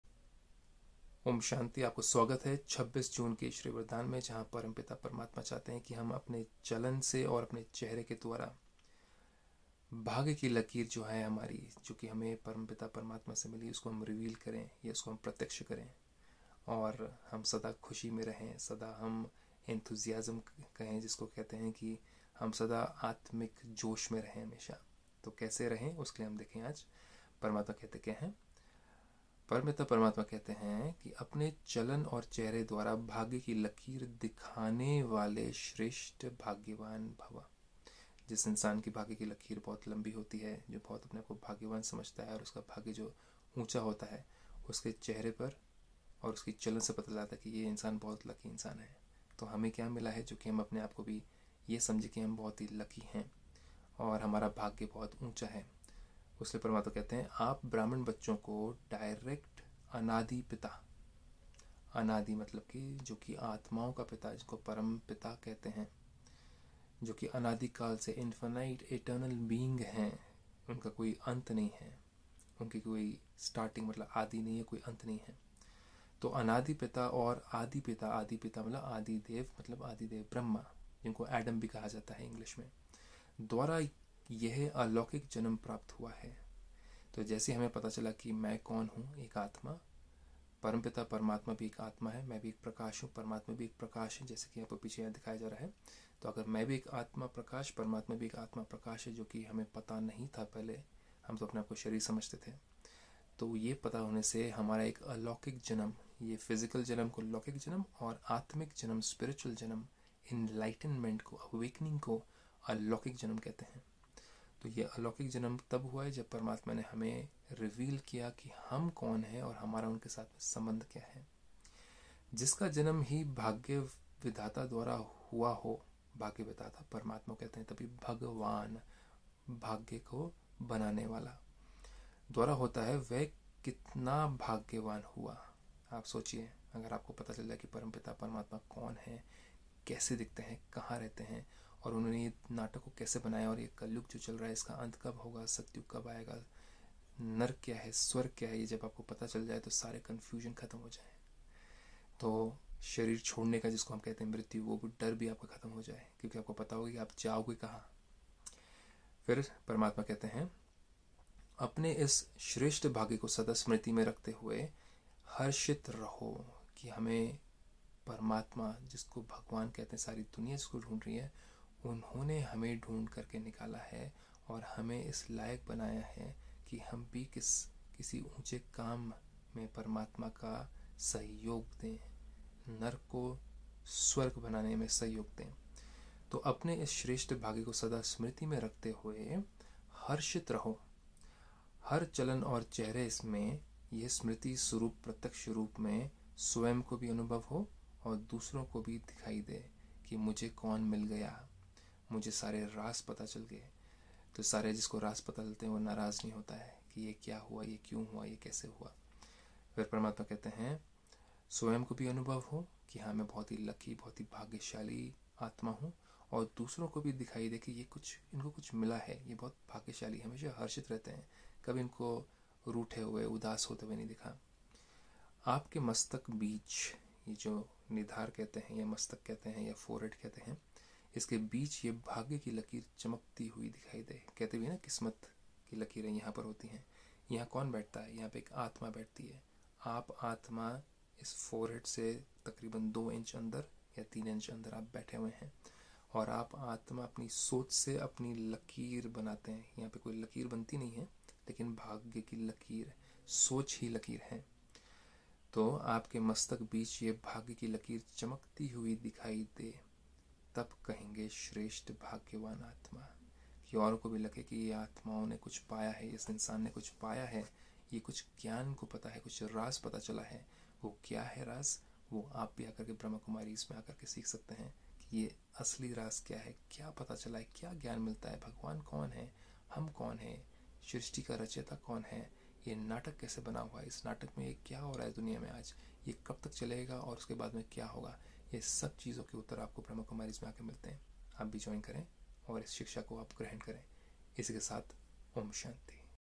सर्वोच्च शाश्वत निर्माता द्वारा दिए गए नए आध्यात्मिक जन्म को याद करने से महान भाग्य का एहसास होता है
दिन 146 - सर्वोच्च शाश्वत निर्माता द्वारा हमें दिए गए नए आध्यात्मिक जन्म को याद करने से एक महान भाग्य का एहसास होता है।

ओम शांति
दिव्य प्रेम परम